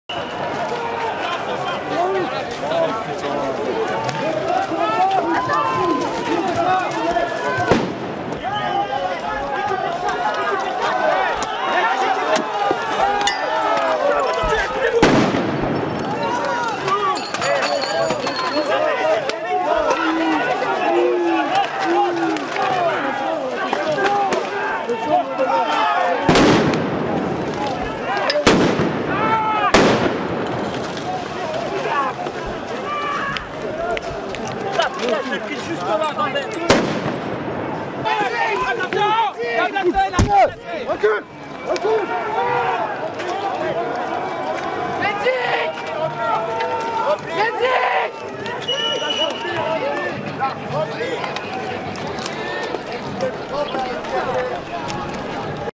First Arrests And Injuries On May Day In Paris
May 01, 2019 - One person is arrested by the police in the midst of clashes near the restaurant La Rotonde, boulevard du Montparnasse in Paris, while another fell to the ground wounded by a projectile. IMAGES